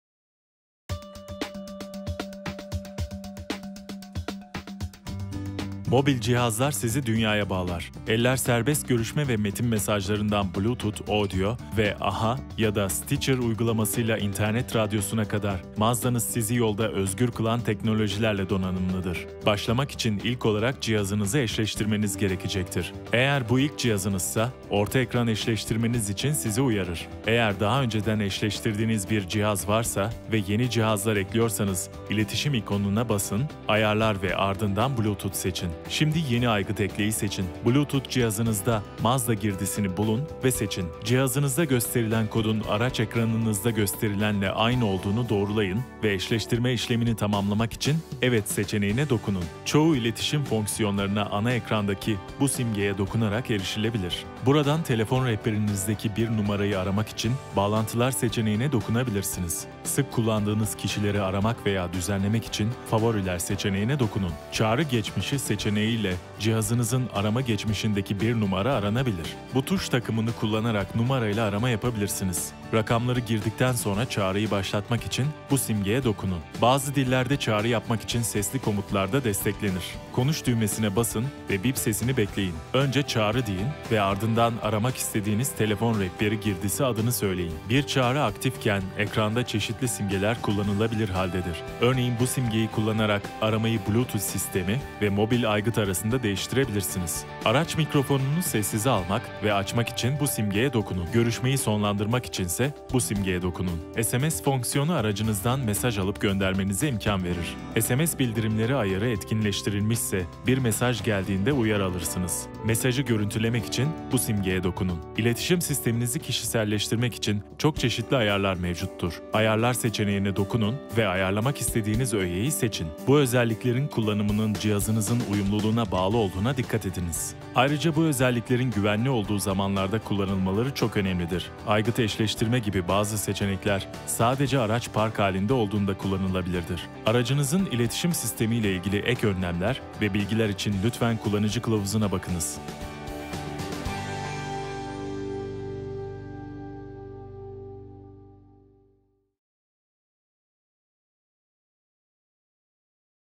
Mazda İletişim
Mobil cihazlar sizi dünyaya bağlar. Elleri kullanmadan arama yapma ve kısa mesaj göndermeden Bluetooth® üzerinden canlı müzik dinlemeye kadar. Uyumlu cihazlarınızı aracınızla nasıl eşleştireceğinizi öğrenin ve Aha ve Stitcher uygulamaları da dahil olmak üzere, Mazda'nın teknolojik açıdan gelişmiş sistemlerinden fazlasıyla yararlanın.

Mazda hakkında daha detaylı bilgi almak için web sitemizi ziyaret edebilirsiniz.